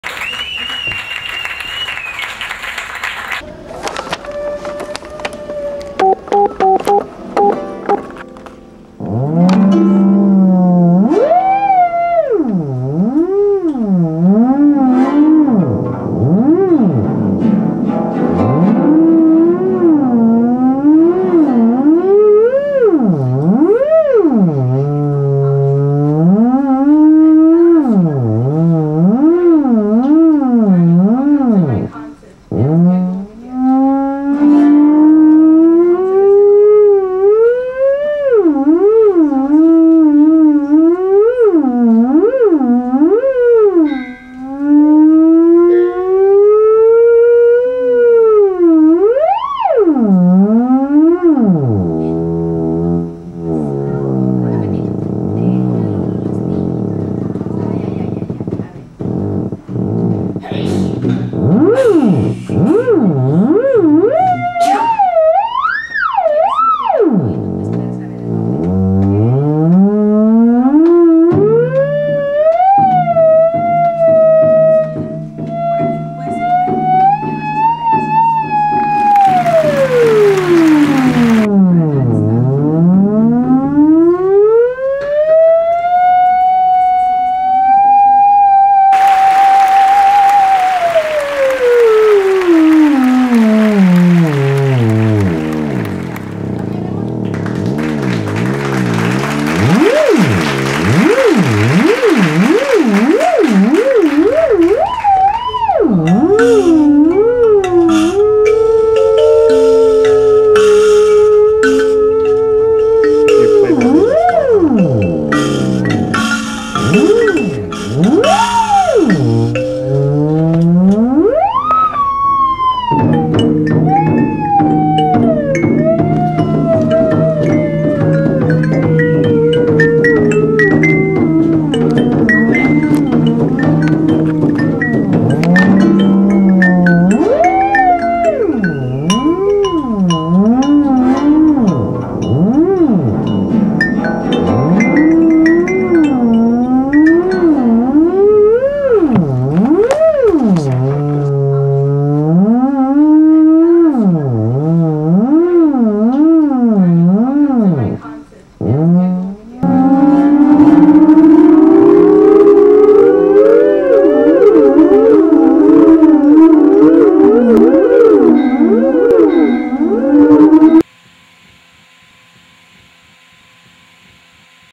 Yes, that's it's title. It appears in the movie. The music was recorded live in the use the instruments room at a Phoenix Music Museum.